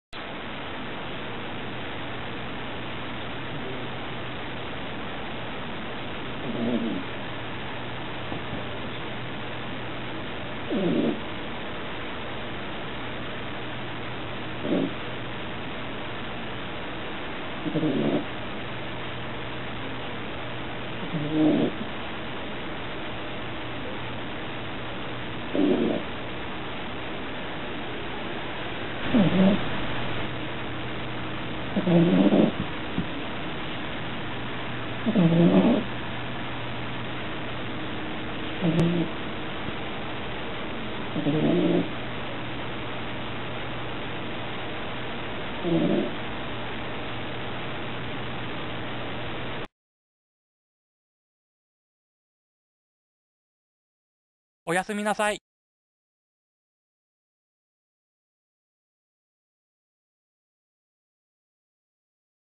でかくない？ナナ・・・ Not bigger? Nana
いびき、でかくない？ナナ・・・
ナナは4匹いるウチの子でイチバンいびきが大きいんです。。。
あはは、パパよりでかいかも（笑）